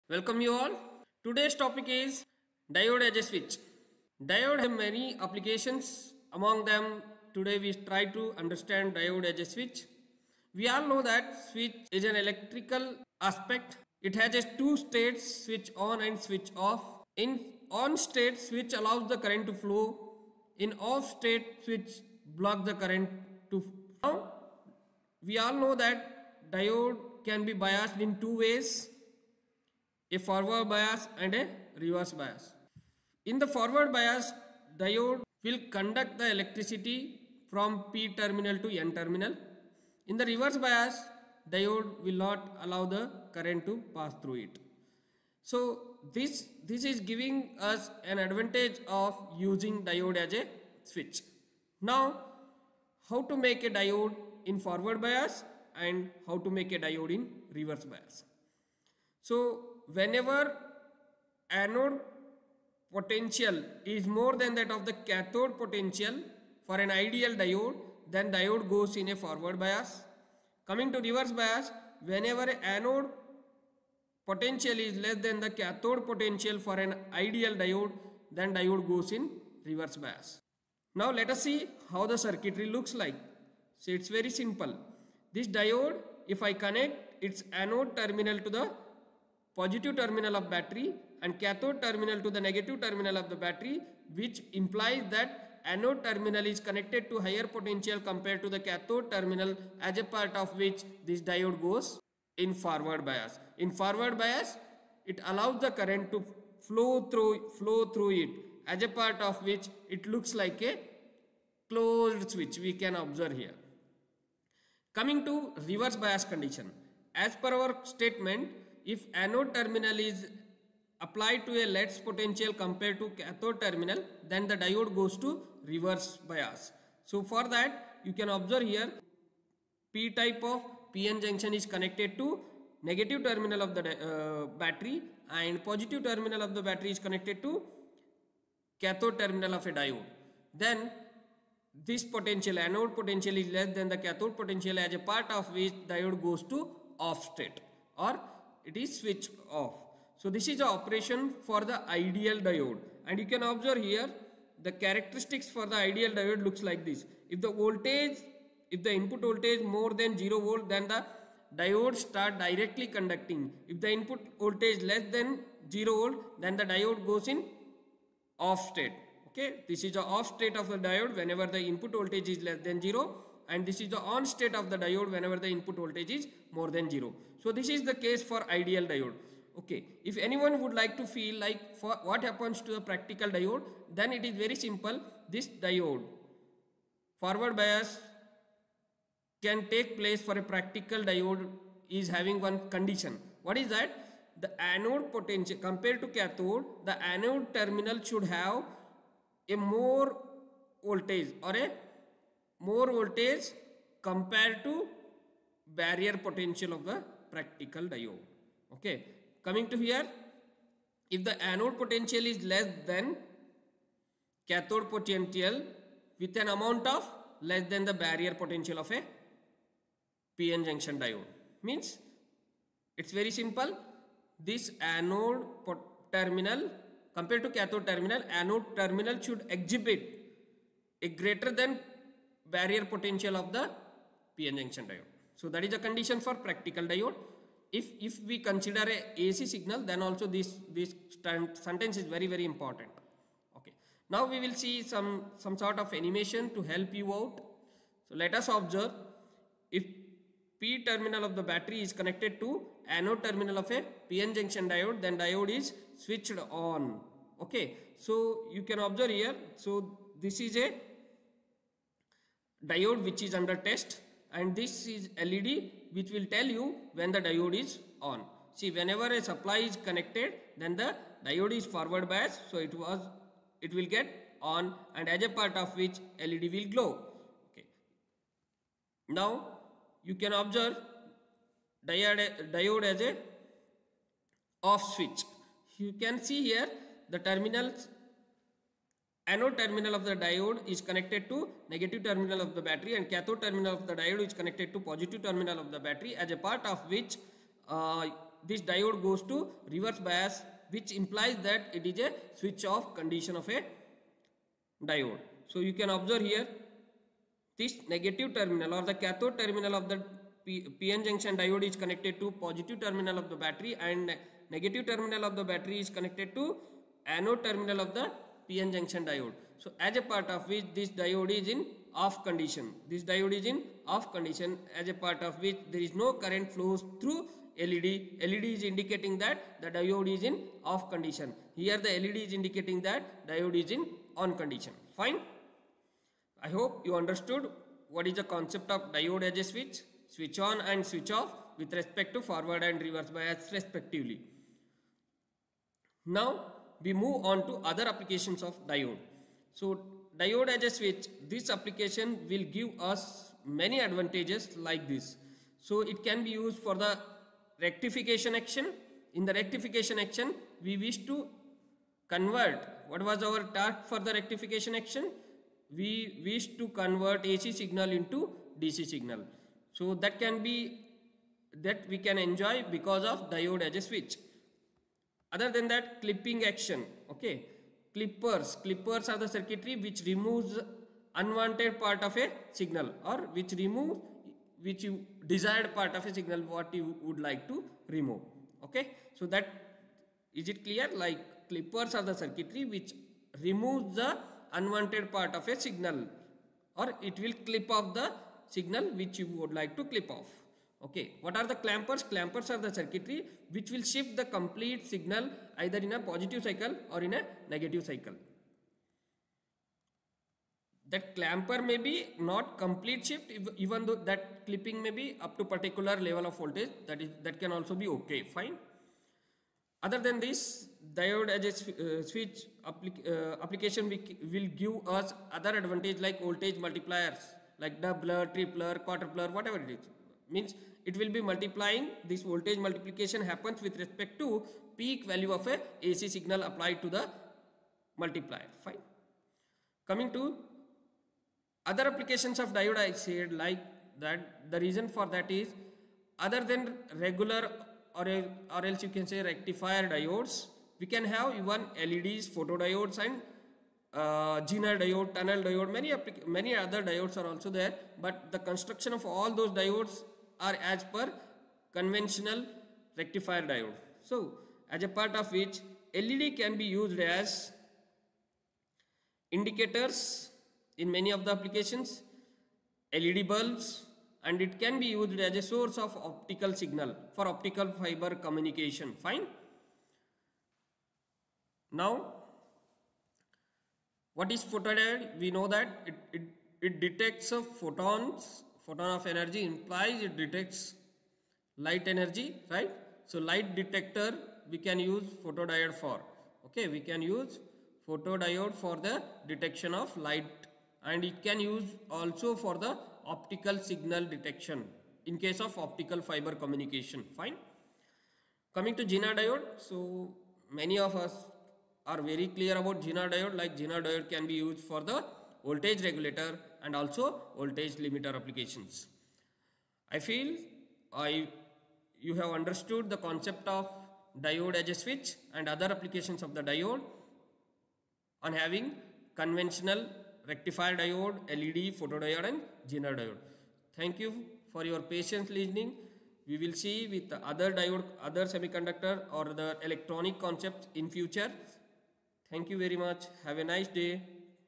Diode as a Switch / Diode / Diode Biasing /Forward Bias / Reverse Bias
Diode Application: Diode as a Switch